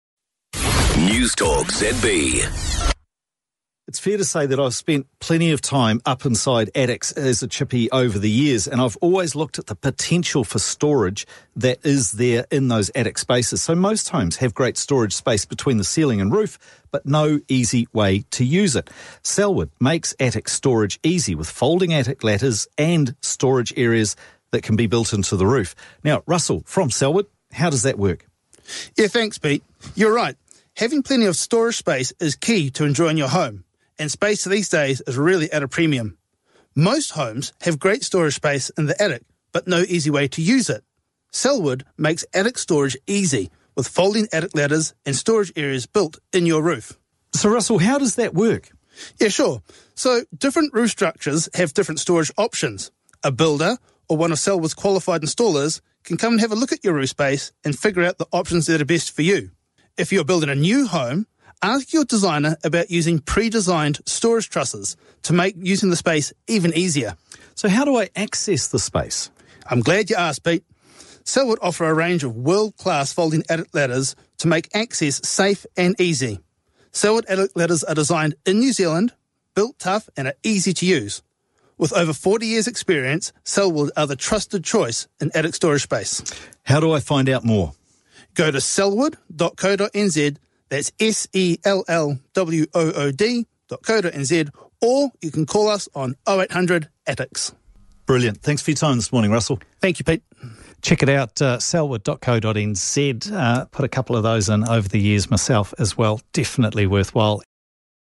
Peter Wolfkamp talks to Russell from Sellwood
The Resident Builder, Peter Wolfkamp talks to Russell from Sellwood.